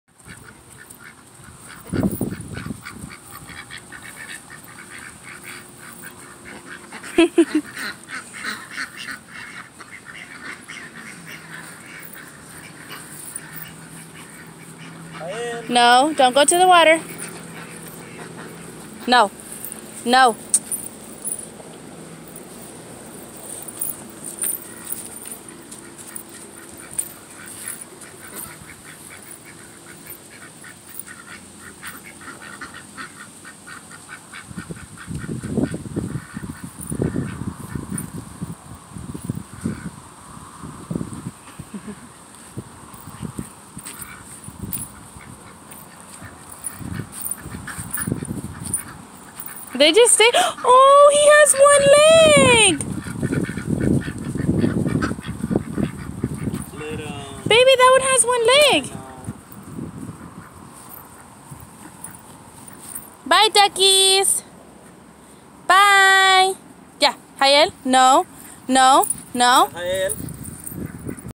Chasing ducks at the riverwalk
Daddy's birthday at the park
Jayel 1y 6m